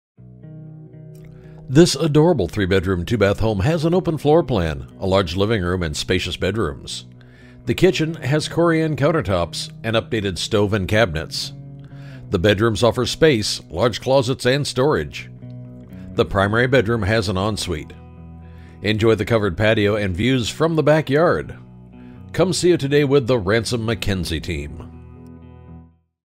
24791 Redcliff Court Moreno Valley, CA
24791 Redcliff Court, Moreno Valley, CA

This adorable three-bedroom, two-bath 1,248 square foot home has been lovingly cared for by the original owners since 1971. The open floor plan for this single-story home offers updated double pane windows, spacious bedrooms, and newer kitchen updates. As you enter the home, the large living room we